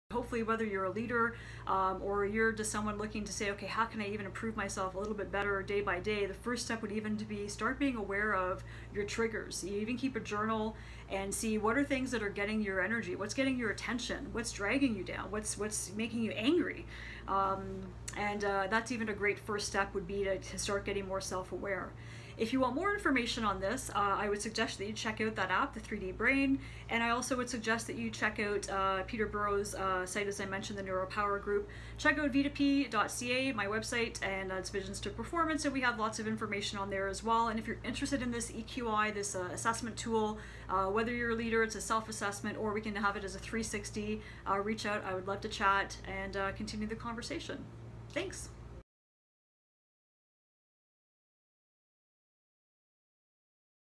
Charmaine Moules: Resources on How to Be More Emotionally Intelligent
In this video I offer some quick resources on how to be more emotionally intelligent.